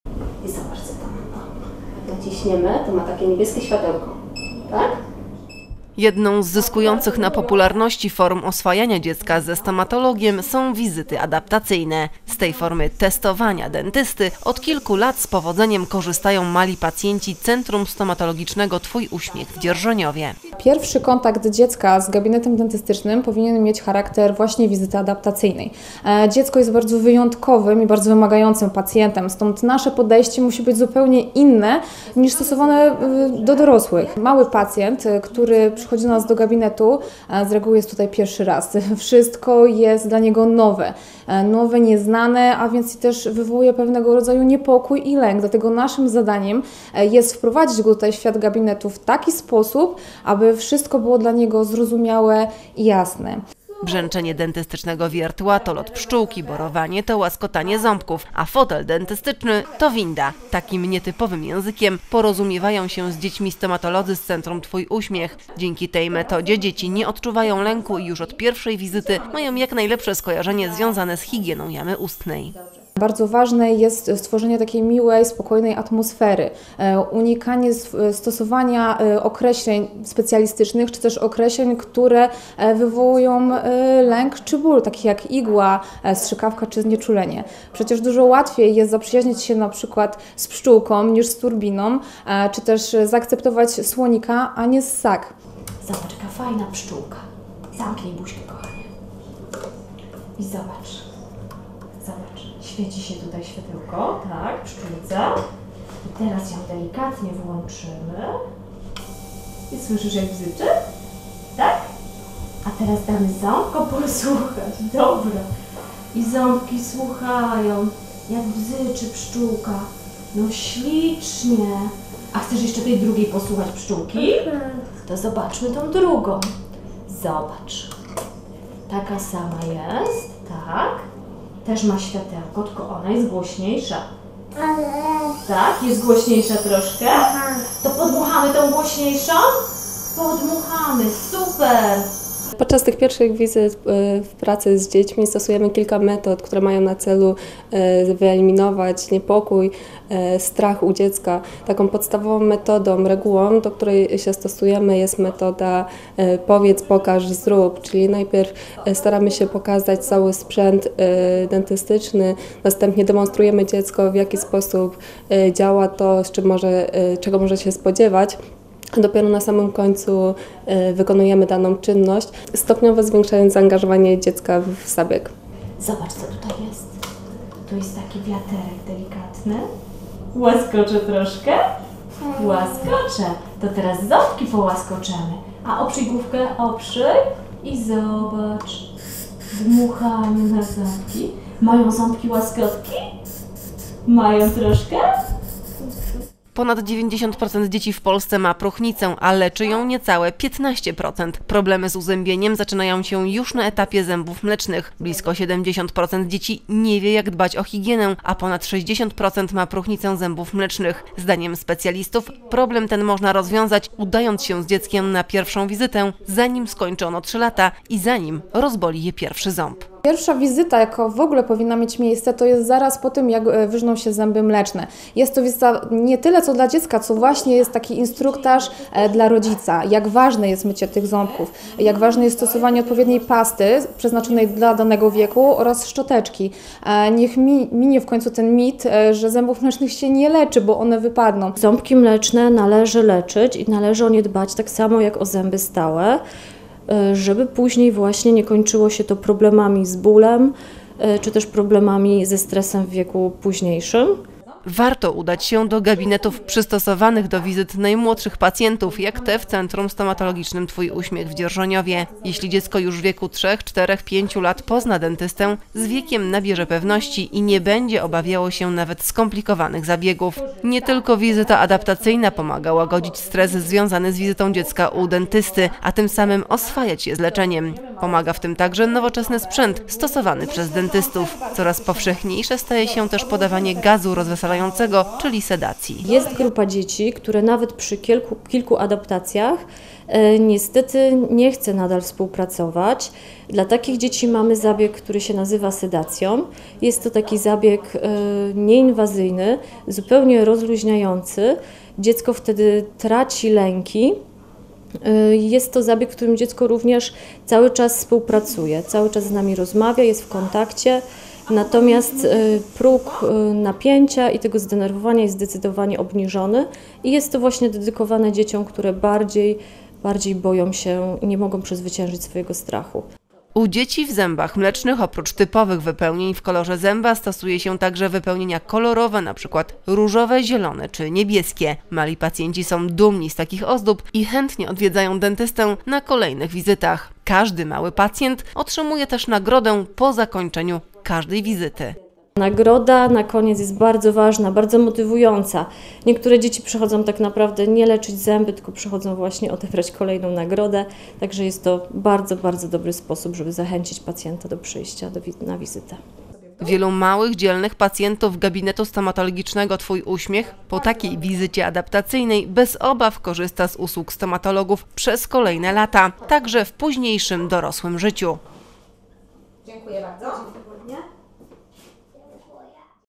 SKUTECZNE OSWAJANIE DZIECKA Z DENTYSTĄ
Jedną z zyskujących na popularności form oswajania dziecka ze stomatologiem są wizyty adaptacyjne. Z tej formy „testowania dentysty" od kilku lat z powodzeniem korzystają mali pacjenci Centrum Stomatologicznego Twój Uśmiech w Dzierżoniowie.